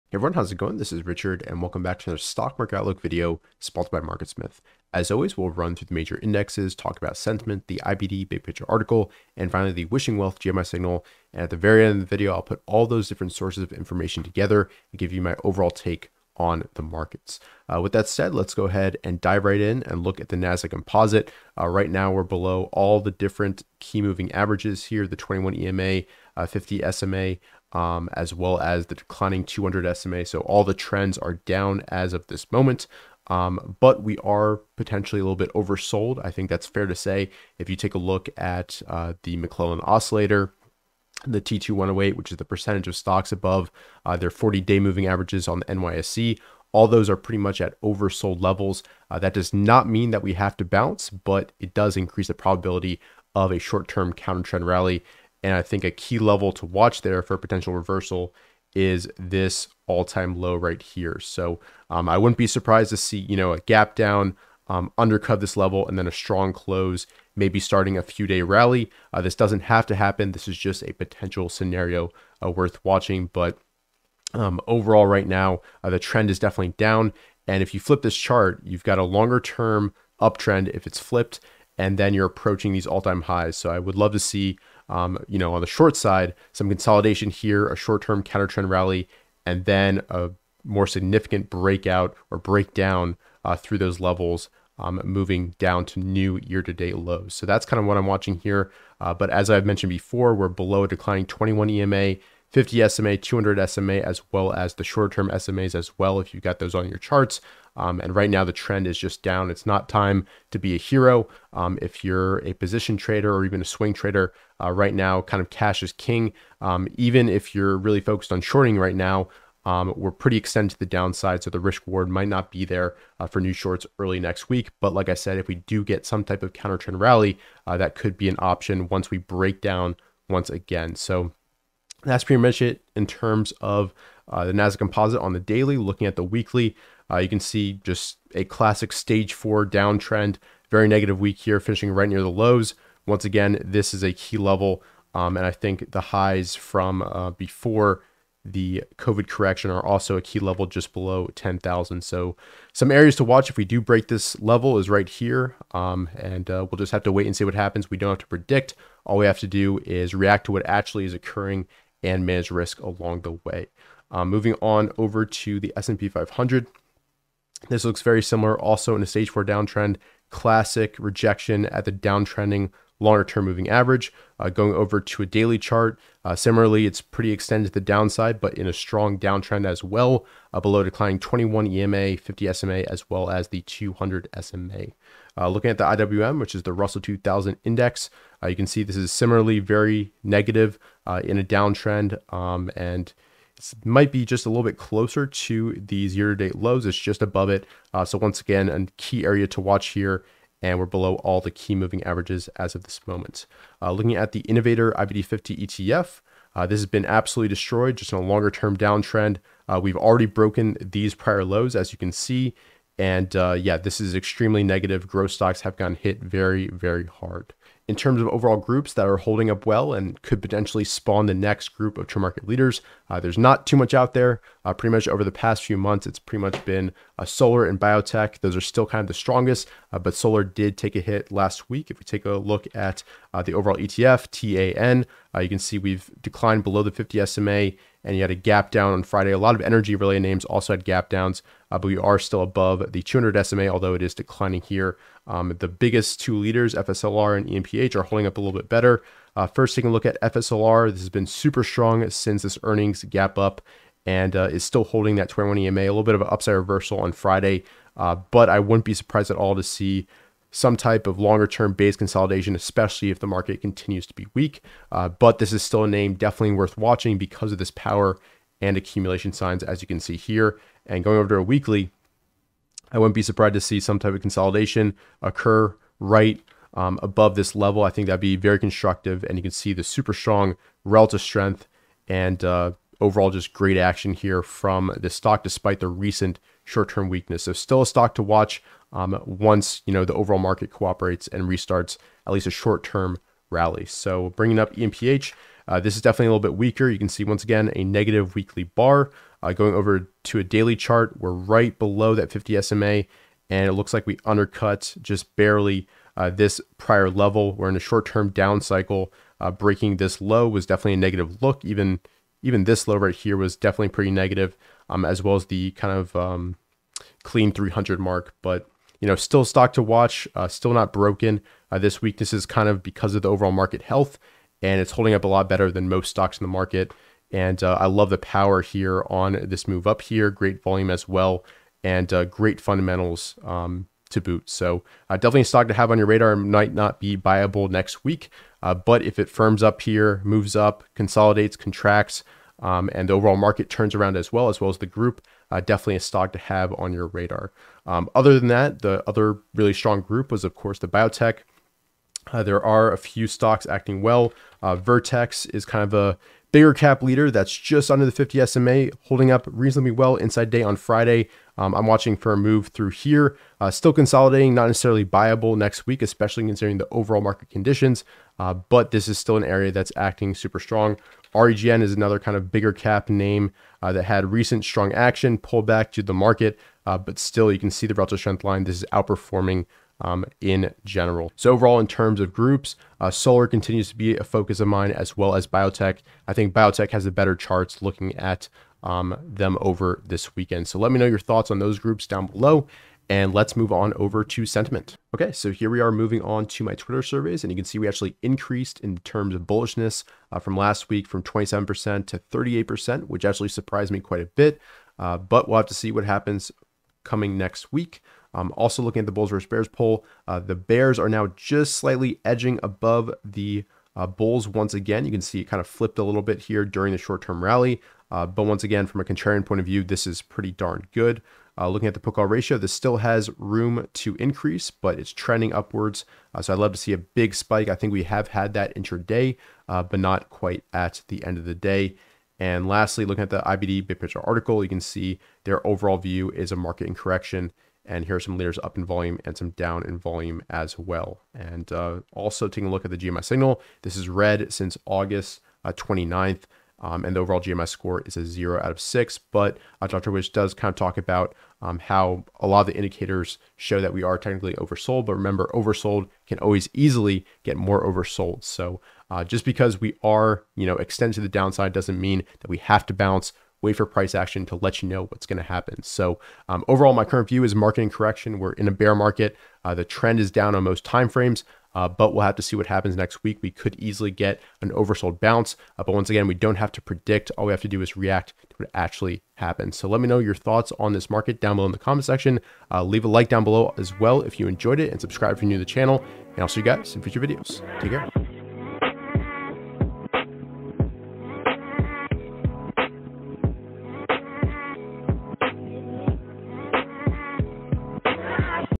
Will the Stock Market Make New Lows??
Welcome to this Week's Stock Market Outlook. I discuss the price action of the major indexes, some top RS stocks, and what I'm looking for next week

The stock market is currently in a downtrend and is technically oversold. however what appears oversold can always deteriorate further so be ready for anything.